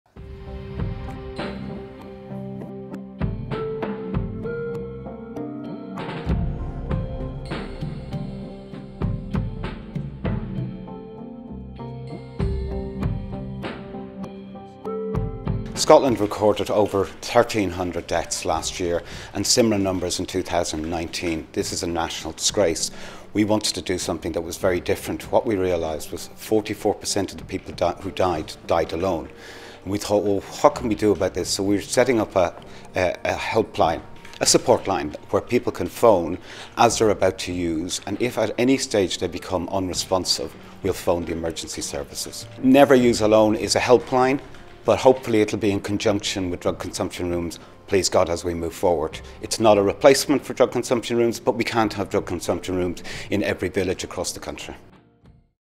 Free helpline for drug users set to launch this month
A free phone service where staff will stay on the line with people who are using drugs is to launch this month.

The anonymous helpline, called “Never Use Alone”, is being trialled in Glasgow, North Ayrshire and South Ayrshire before being rolled out across Scotland.

It will be run by the charity We Are With You, who say they will focus only on the safety of drug users.

The helpline has been backed with £100,000 from the Scottish Government.